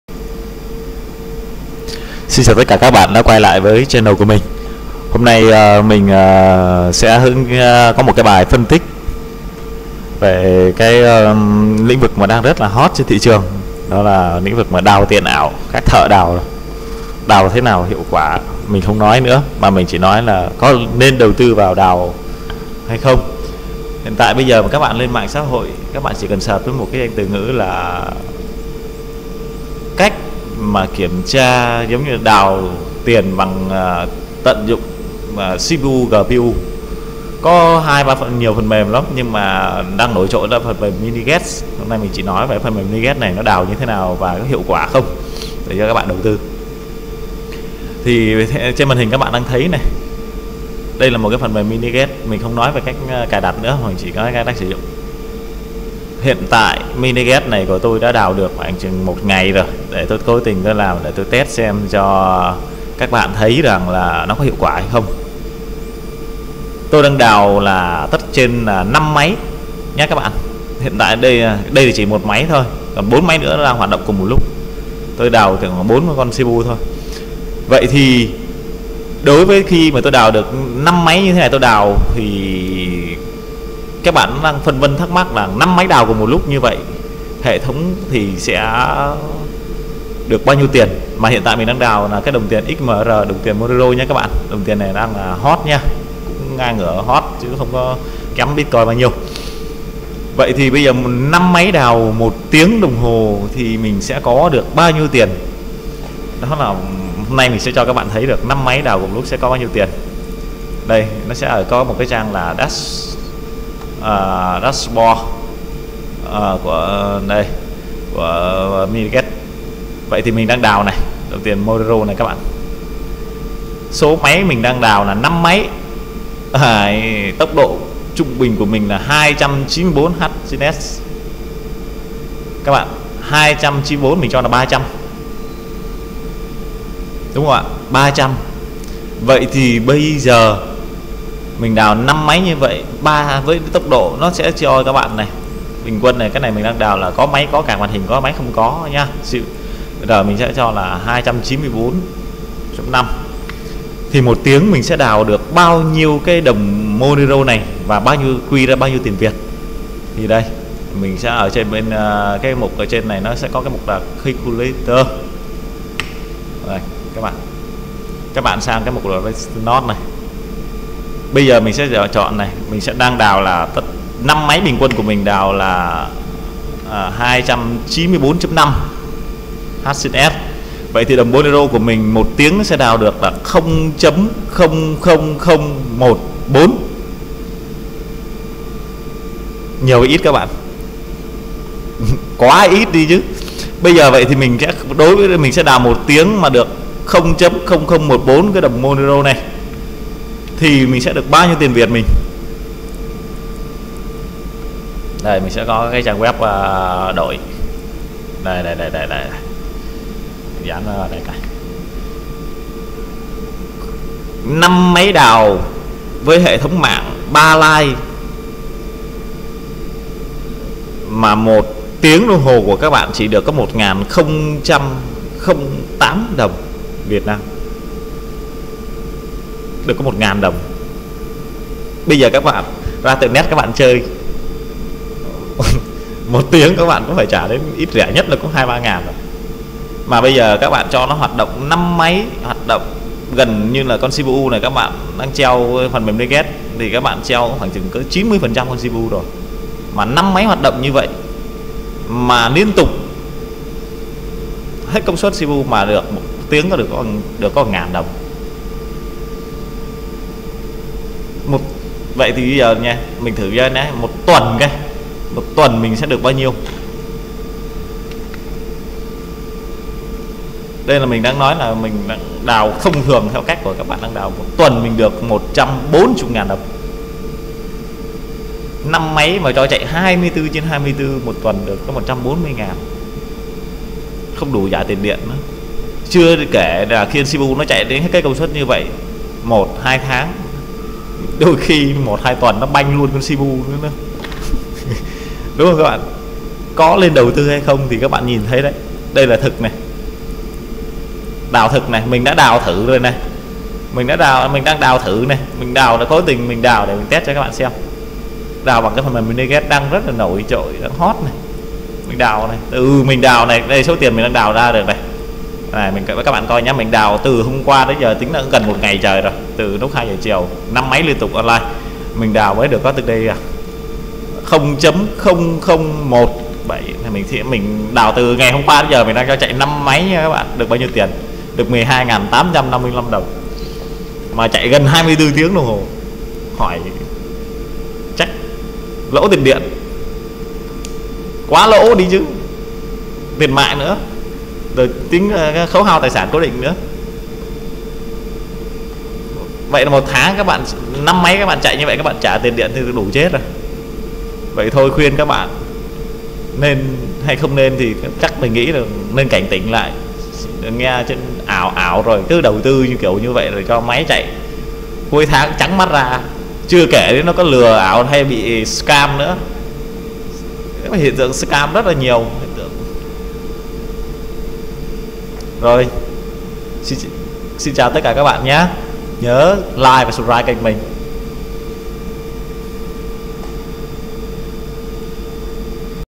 Đào tiền điện tử bằng Minergate có thực sự lợi nhuận
Video phân tích đào tiền điện tử thông qua tool minergate có thực sự hiệu quả như những trang web quảng cáo thổi phồng.
Treo máy là có tiền.